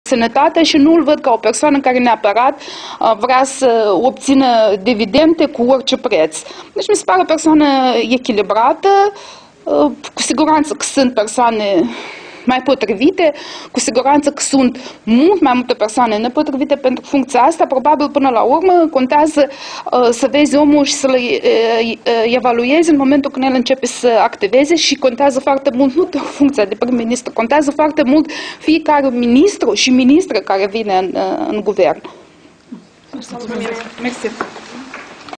Declarațiile Antoniței Fonari după consultările prim-ministrului desemnat, Pavel...
Declarațiile Antoniței Fonari după consultările prim-ministrului desemnat, Pavel Filip, cu reprezentanții societății civile.*Antonita Fonari ne reprezinta societatea civila...
Titlul evenimentului: Declarațiile Antoniței Fonari după consultările prim-ministrului desemnat, Pavel Filip, cu reprezentanții societății civile